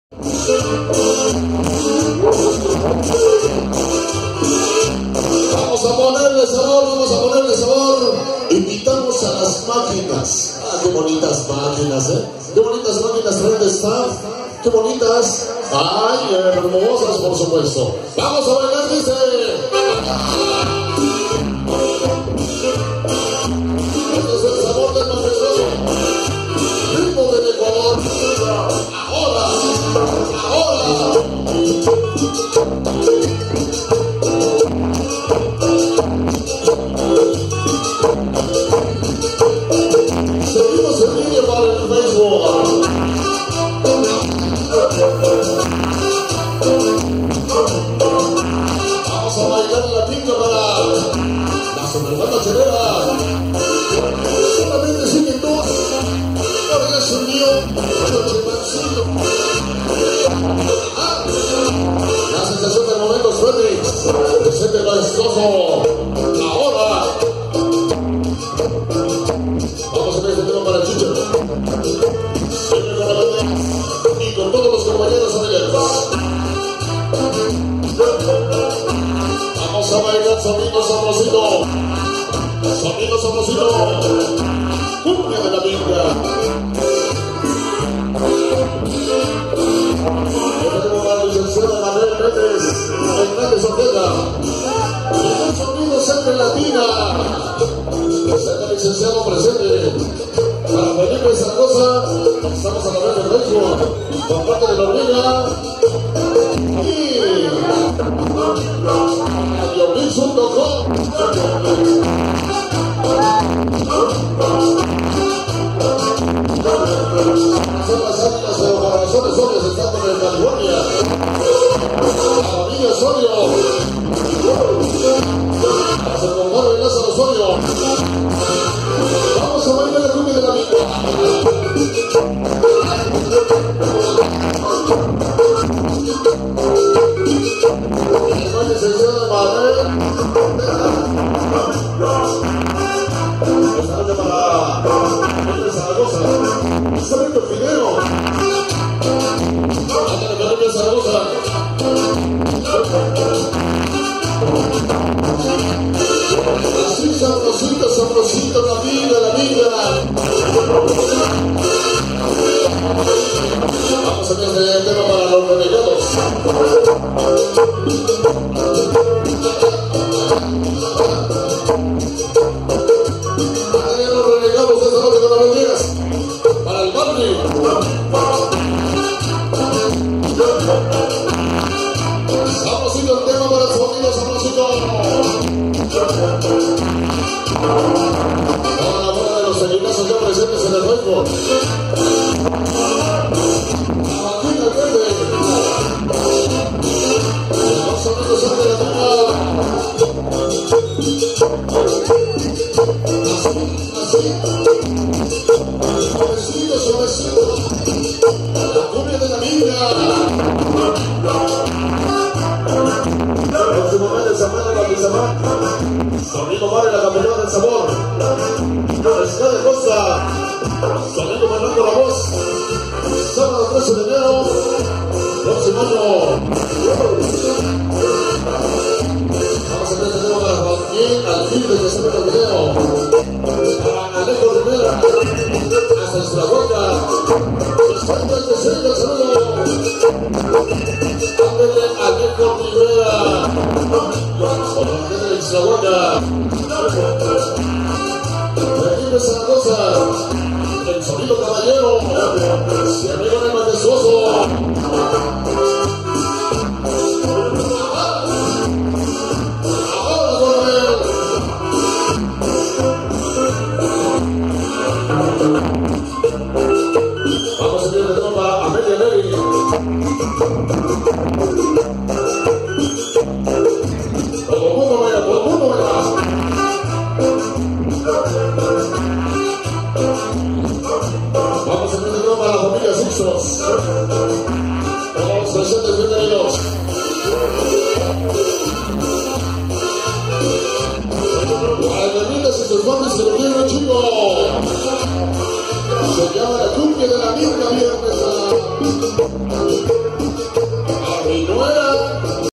Sonido Avel Diaz-la minga 2017
Desde San Miguel Chapultepec El majestuoso Avel Diaz en San Pedro tlaltizapan